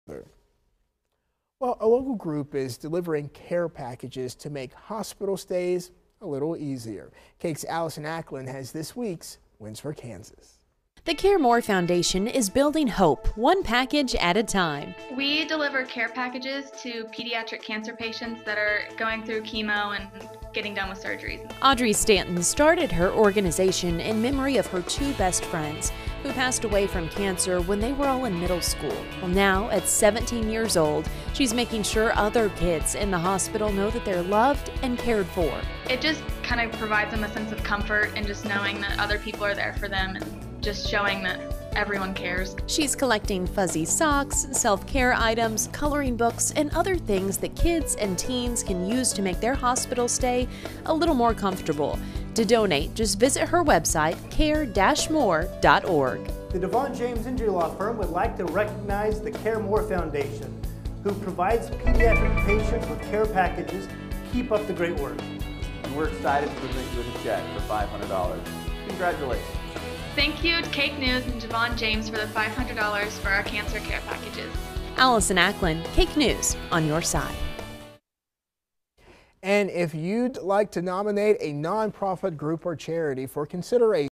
Care More Foundation - DeVaughn James Injury Lawyers WINS for Kansas
Care More Foundation is focused on delivering hope and comfort to children and their families battling cancer.